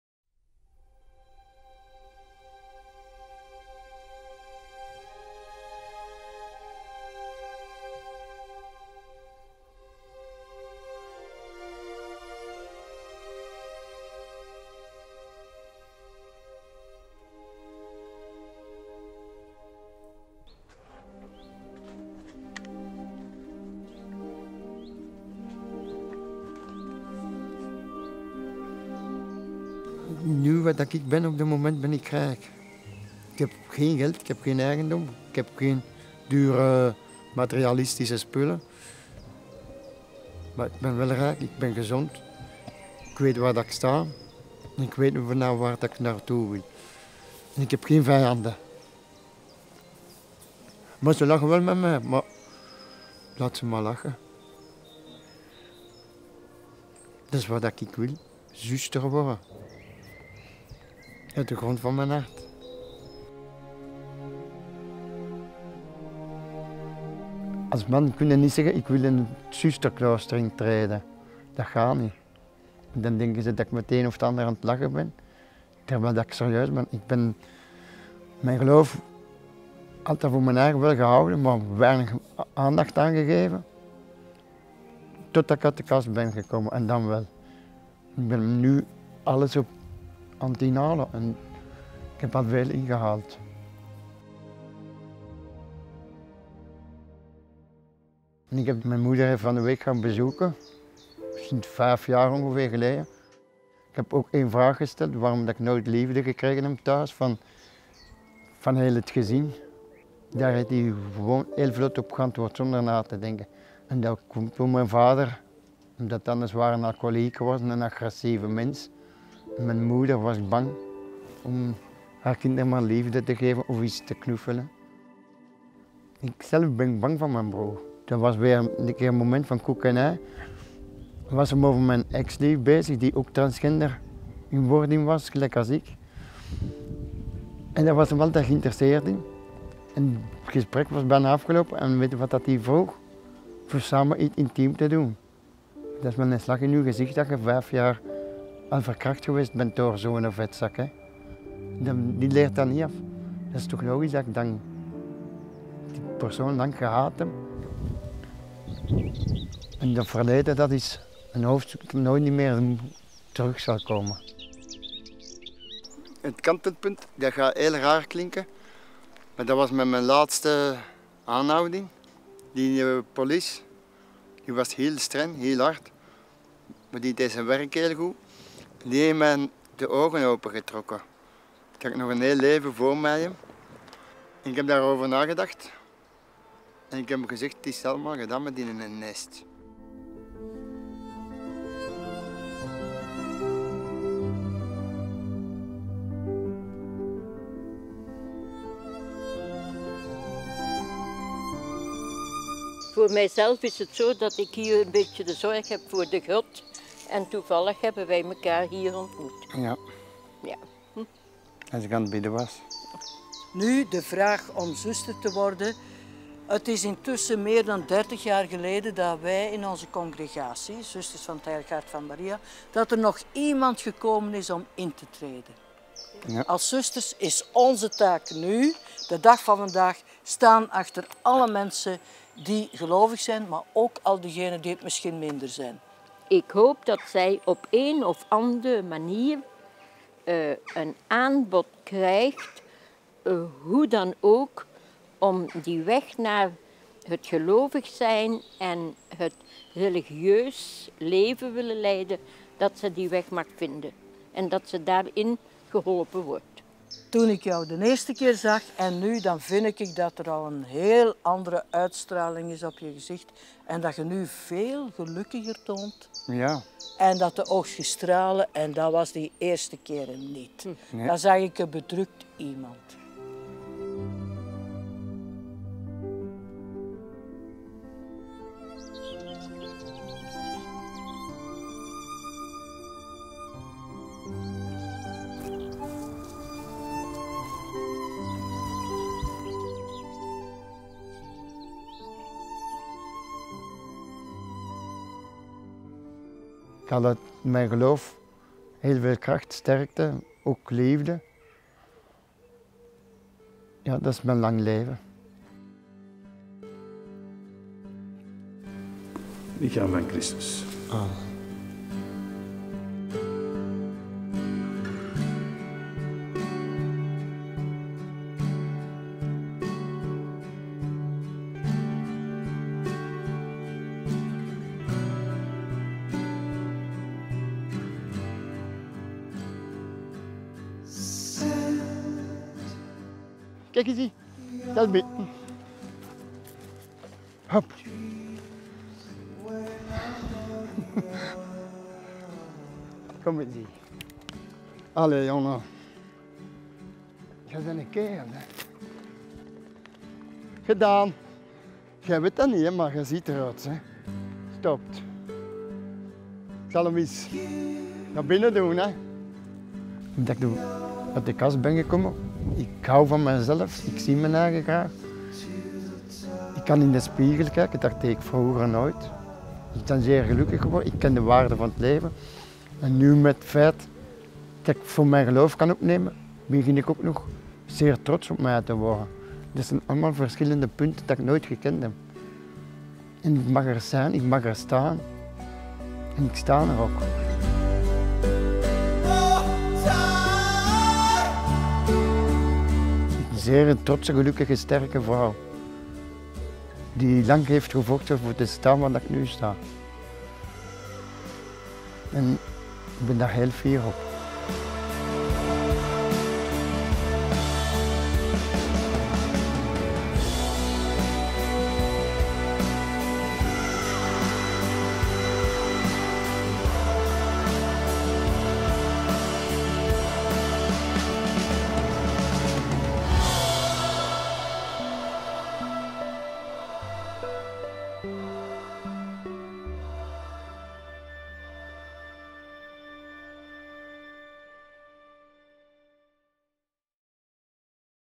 Zuster Eefje - Reportage
Zuster Eefje is een transgendervrouw die het klooster wil toetreden als Zuster.
Via deze weg kan je haar verhaal leren kennen.

Our channel is about filmmaking. We post lots of cool projects such as Shortfilms, Aftermovies and Commercials